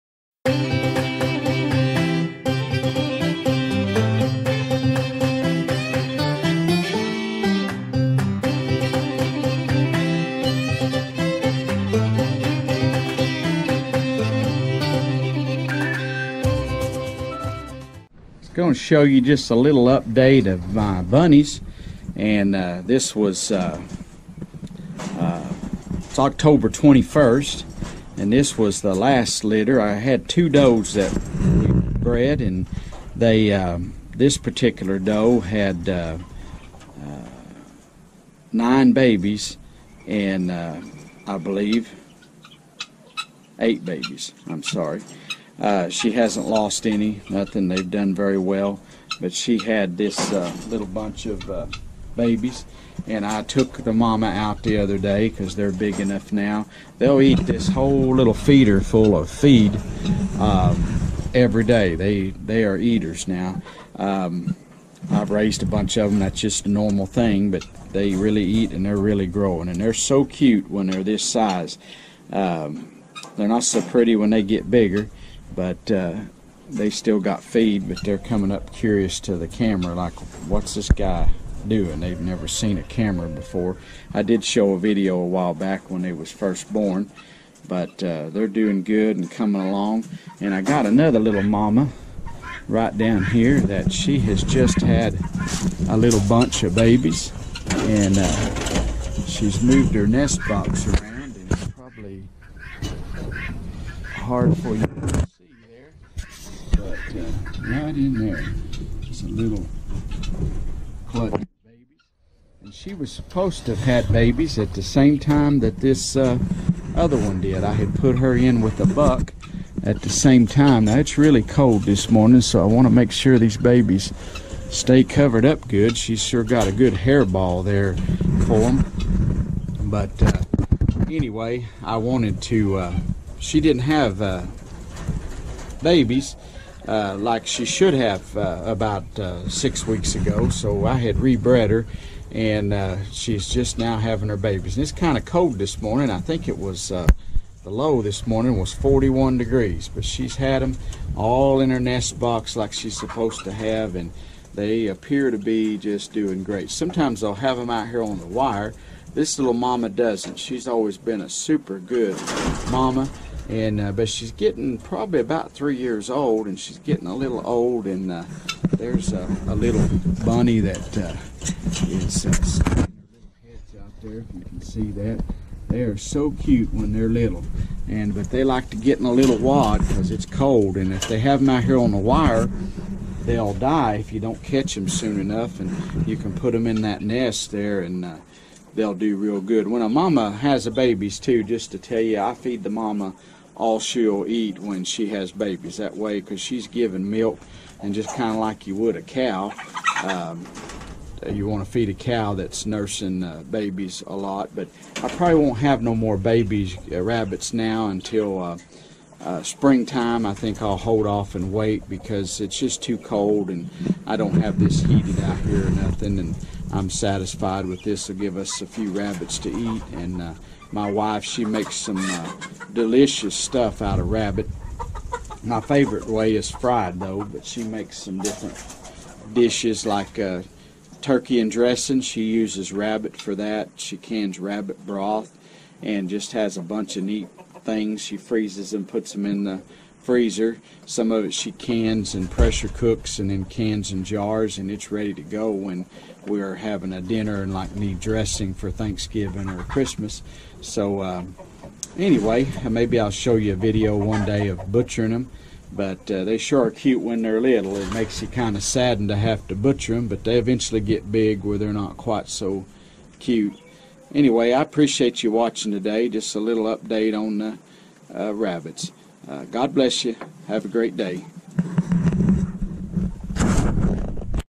Raising Rabbits on the homestead for fun and food.
A short video about our raising rabbits on the Homestead